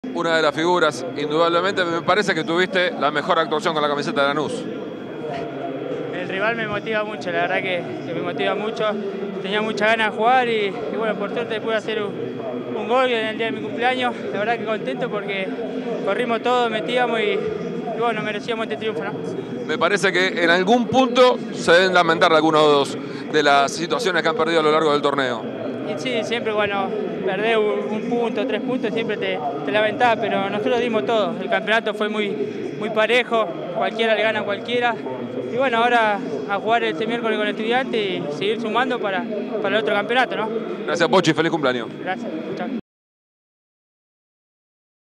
Chávez "El rival me motiva mucho".
El Pochi Chávez analiza el triunfo ante River por 5 a 1.